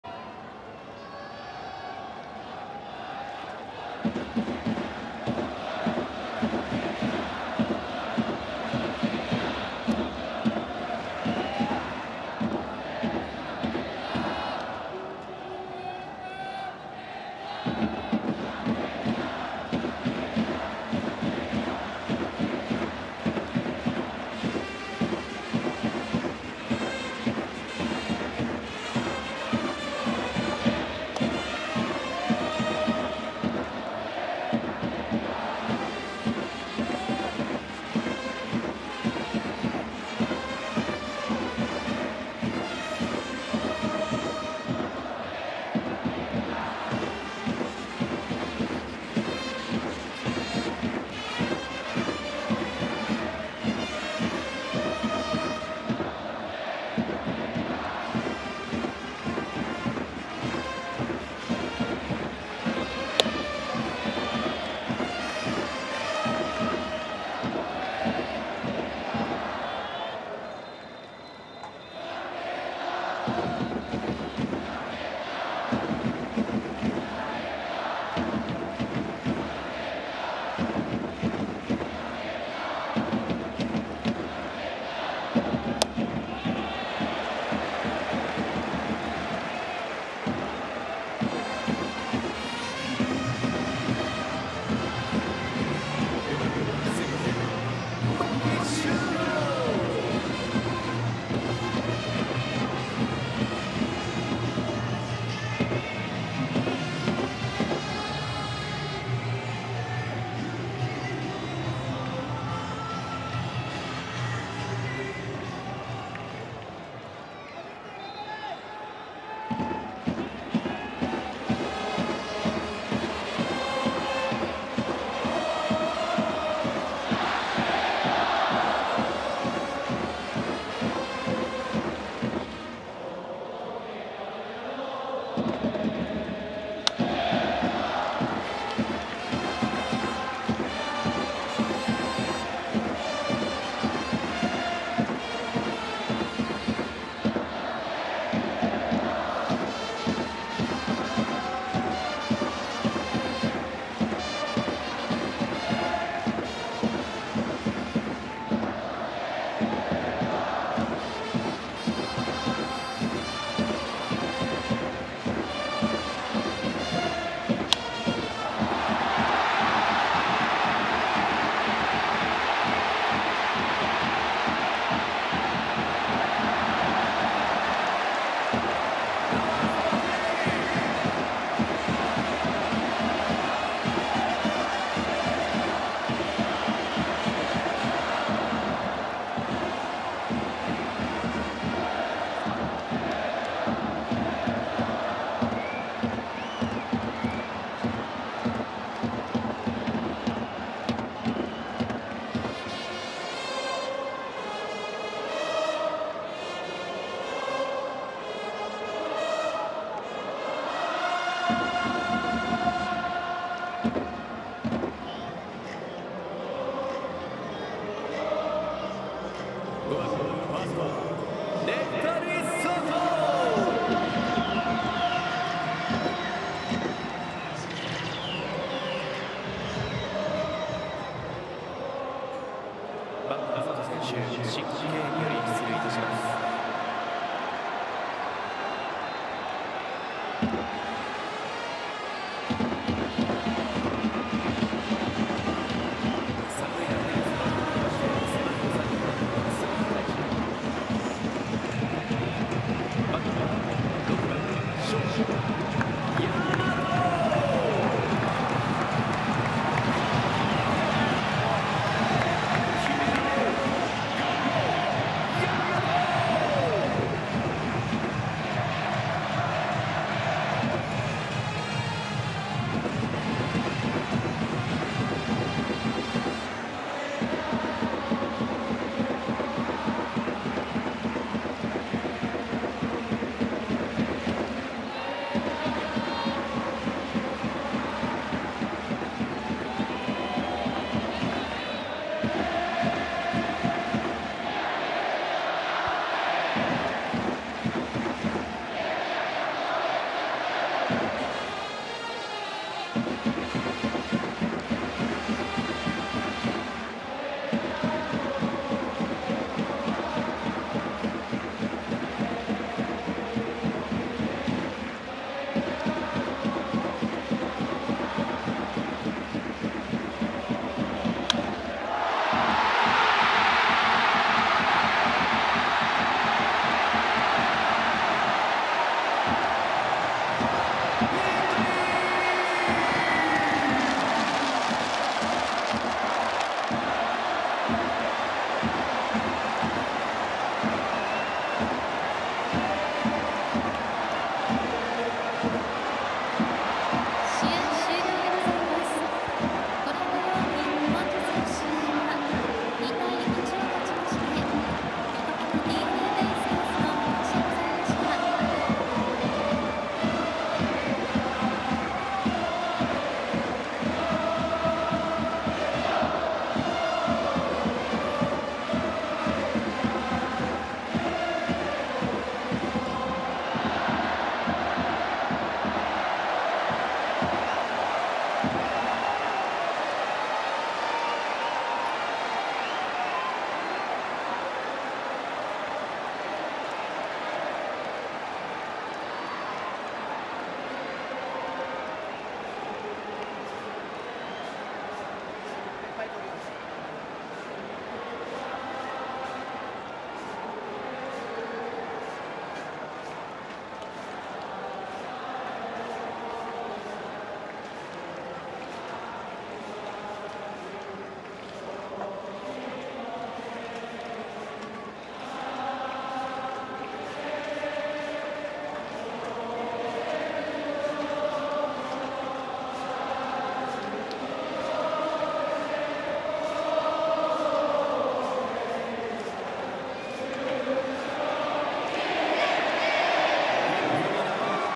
【延長戦】10回裏のサヨナラ勝利をノーカットでお届け！｜2023.9.8の注目シーン
注目選手、注目シーンをピックアップ。

サヨナラノーカット！！
大和 選手は2年ぶりのサヨナラ打！